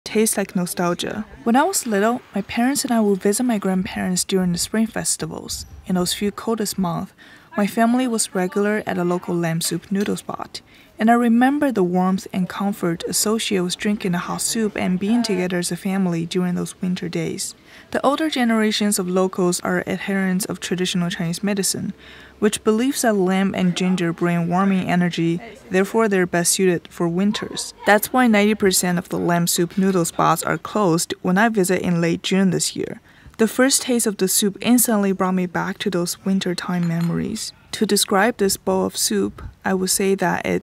Tastes Like Nostalgia - Lamb Noodle Soup
Growing up, having this bowl of hot lamb noodle soup has become a norm for winter holidays. My parents and I lived in the northern part of China while my grandparents were down south in Su Zhou. Every time we visited them during the spring festivals, our entire family would get the lamb noodle soup once every few days. As the spring festival was celebrated nationally, most of the restaurants were closed during the holiday - these noodle spots were perhaps the only few opened. Not only was it convenient as a quick meal, the hot lamb soup, and warming ginger were the best remedy for a freezing cold winter day.

I haven't been to Su Zhou in almost 6 years yet I still long for this bowl of noodle soup regularly. It really tastes phenomenal - the slow-cooked lamb soup is super flavorful and the noodles (they're a local special) are perfectly al dente. But more than that, its tastes bring back some favorite memories of mine being together with my family for the holidays. And just the simple pleasure of enjoying a hot soup while it's minus 30 degrees outside was memorable by itself.

This time, I visited Su Zhou in late June. Locals are adherents of traditional Chinese medicine so the majority of the lamb soup noodle restaurants were closed during my visit yet I found this one on DianPing (equivalent to yelp). It was so good, just like how I remembered it and how I've been imagining it in my dreams lol.

Memories are formed through the culmination of experiences that includes all 5 senses. What are the nostalgic tastes special to you?